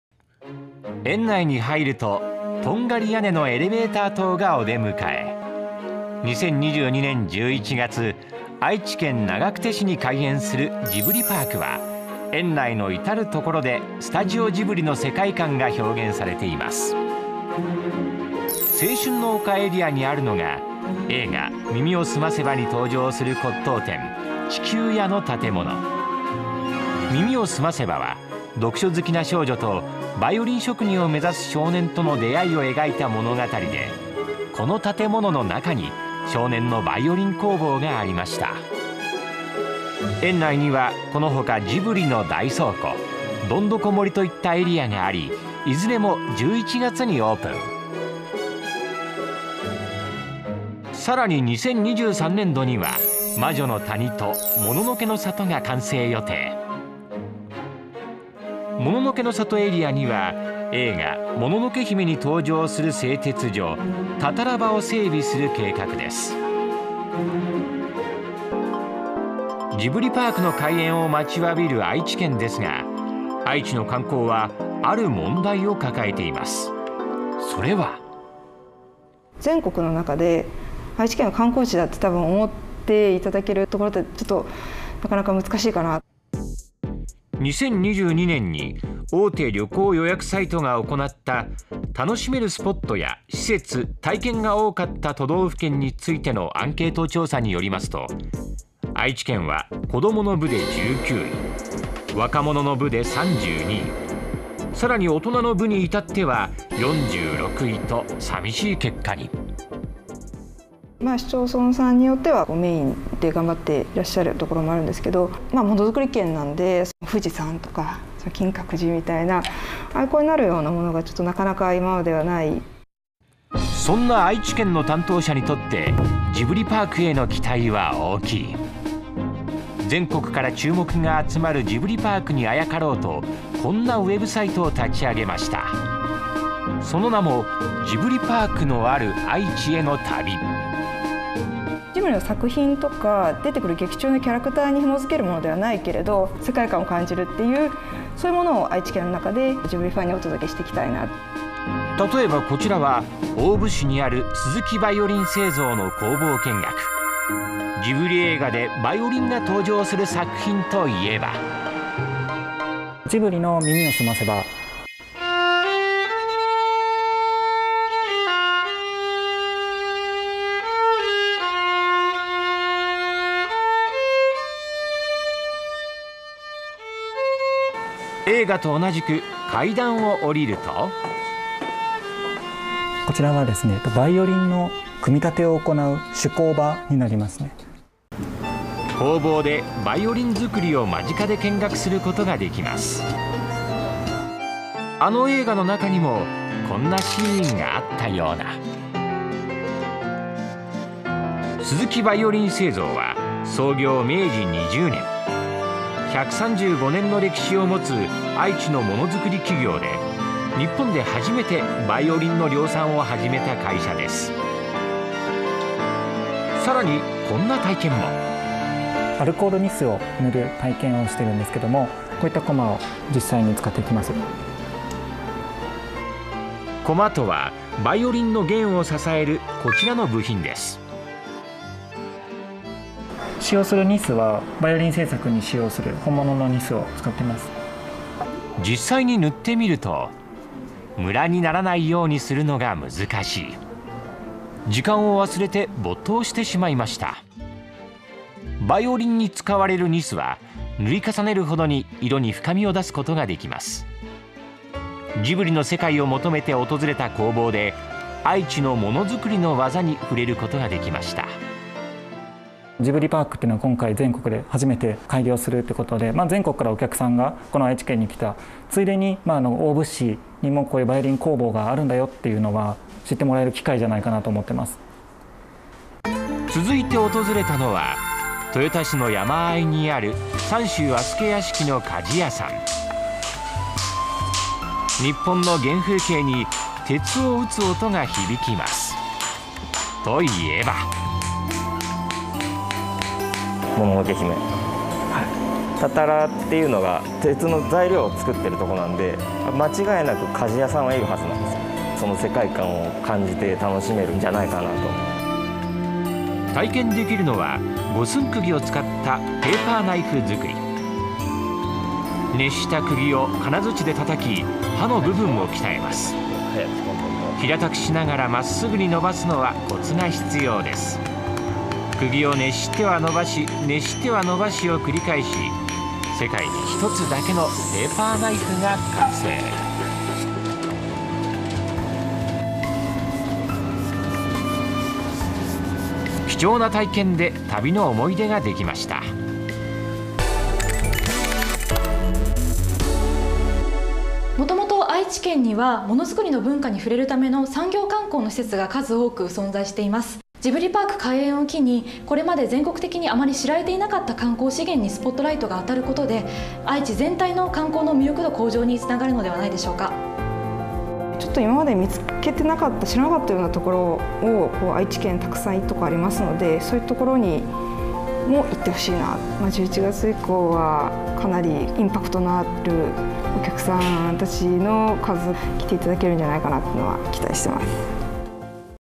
【ジブリの世界観（LBS名古屋）】ジブリパークにあやかりたい！愛知の観光戦略
ローカルビジネスサテライトナゴヤ限定版です。
今年11月に開園する「ジブリパーク」。観光地だと思われにくい愛知県にとっては期待の星です。注目が集まるジブリパークにあやかろうと、様々な取り組みが行われています。

▼内容
園内に入ると、とんがり屋根のエレベーター塔がお出迎え。
2022年11月、愛知県長久手市に開園する「ジブリパーク」は園内のいたるところでスタジオジブリの世界感が表現されています。

「青春の丘」エリアにあるのが、映画「耳をすませば」に登場する骨董店「地球屋」の建物。
耳をすませばは、読書好きな少女とバイオリン職人を目指す少年との出会いを描いた物語で、この建物の中に少年のバイオリン工房がありました。

園内には、このほか「ジブリの大倉庫」、「どんどこ森」といったエリアがあり、いずれも11月にオープン。

さらに2023年度には「魔女の谷」と「もののけの里」が完成予定。
特に「もののけの里」エリアは、映画「もののけ姫」に登場する製鉄所、「タタラ場」を整備する計画です。

ジブリパークの開園を待ちわびる愛知県ですが、愛知の観光は、ある問題を抱えています。
それは…

（愛知県 観光振興課 鈴木さん）「全国の中で愛知県を観光地だと思っていただくのはなかなか難しい」

2022年に大手旅行予約サイトが行った、「楽しめるスポットや施設・体験が多かった都道府県についてのアンケート調査」によりますと、愛知県は、子どもの部で19位。若者の部で32位。さらに大人の部にいたっては46位と、さみしい結果に…。

（鈴木さん）「市町村によっては観光メインで頑張っているところもあるが、モノづくり県なので、富士山とか、金閣寺とかアイコンになるようなものが愛知県にはなかなか今までに無い。」

そんな愛知県の担当者にとって、ジブリパークへの期待は大きいです。全国から注目が集まるジブリパークにあやかろうと、こんなウェブサイトを立ち上げました。
その名も、「ジブリパークのある愛知への旅」。

（鈴木さん）「ジブリの作品とか出てくる劇中のキャラクターに紐づけるものではないけど、世界観を感じるというのを愛知県の中でジブリファンに届けたい」

例えばこちらは大府市にある鈴木バイオリン製造の工房見学。
ジブリ映画でバイオリンが登場する作品といえば…

（鈴木バイオリン製造 小野田社長）「ジブリの『耳をすませば』」

映画と同じく建物の一階におりると…

（小野田社長）「こちらはバイオリンの組み立てを行う手工場になります。」

工房で、バイオリンづくりを間近で見学することができます。
あの映画の中にも、こんなシーンがあったような。

鈴木バイオリン製造は創業明治20年。135年の歴史をもつ、愛知のものづくり企業で、日本で初めてバイオリンの量産を始めた会社です。

さらにこんな体験も…

（小野田社長）「アルコールニスを塗る体験をしているんですけど、こういった駒を実際に使っていきます。」

駒とは、バイオリンの弦を支えるこちらの部品です。

（小野田社長）「使用するニスはバイオリン制作に使用する本物のニスを使ってます。」

実際に塗ってみると、ムラにならないようするのがむずかしい…。
時間を忘れて没頭してしまいました。

バイオリンに使われるニスは、塗り重ねるほどに色に深みを出すことができます。
ジブリの世界を求めて訪れた工房で、愛知のものづくりの技に触れることができました。

（小野田社長）「ジブリパークというのが今回全国で初めて開業することで、全国から客が愛知県に来るついでに、大府市にもこういったバイオリン工房があることを知ってもらえる機会では。」

つづいて訪れたのは、豊田市の山間にある三州足助屋敷の鍛冶屋さん。
日本の原風景に、鉄を打つ音が響きます。
といえば…

（広瀬重光刃物店 廣瀬さん）「もののけ姫。たたらというのは鉄の材料をつくっているところなので、間違いなく鍛冶屋さんはいるはず。その世界観を感じて楽しめるんじゃないかな」

体験できるのは五寸釘を使ったペーパーナイフづくり。
熱した釘を金槌で叩き、刃の部分を鍛えます。
平たくしながらまっすぐに伸ばすのは、コツが必要です。
釘を熱しては伸ばし、熱しては伸ばしを繰り返し、世界に一つだけのペーパーナイフが完成。
貴重な体験で、旅の思い出ができました。

（日本経済新聞 門岡記者）「もともと愛知にはものづくりの文化に触れる産業観光の施設が数多くある。ジブリパークの開業で、全国的にあまり知られていなかった隠れた観光資源にスポットライトがあたるようになれば、愛知全体の観光の魅力度アップにつながるのではないか？」

（鈴木さん）「今まで見つけていなかった知らなかったようなところ、愛知県たくさんいいところありますのでそういうところにも行ってほしいな。11月以降はかなりインパクトのある観光客数が来てくれるんじゃないかと期待している。」